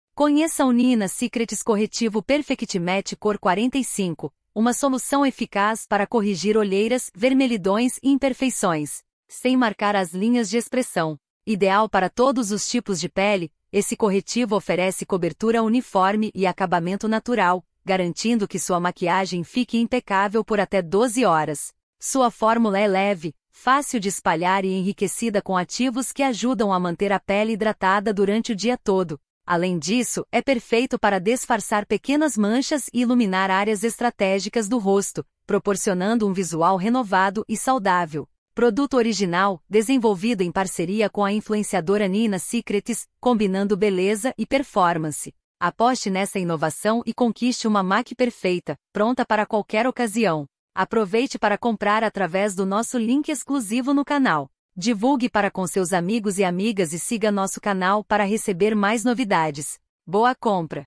Niina Secrets Corretivo Perfect Match: Corrija Olheiras e Renove Seu Visual!
Conheça o NIINA SECRETS Corretivo Perfect Match Cor 45, uma solução eficaz para corrigir olheiras, vermelhidões e imperfeições, sem marcar as linhas de expressão. Ideal para todos os tipos de pele, esse corretivo oferece cobertura uniforme e acabamento natural, garantindo que sua maquiagem fique impecável por até 12 horas.

Sua fórmula é leve, fácil de espalhar e enriquecida com ativos que ajudam a manter a pele hidratada durante o dia todo. Além disso, é perfeito para disfarçar pequenas manchas e iluminar áreas estratégicas do rosto, proporcionando um visual renovado e saudável.

Produto original, desenvolvido em parceria com a influenciadora Niina Secrets, combinando beleza e performance. Aposte nessa inovação e conquiste uma make perfeita, pronta para qualquer ocasião!

Compartilhe com suas amigas e siga nossa página para receber as novidades.

Boa compra!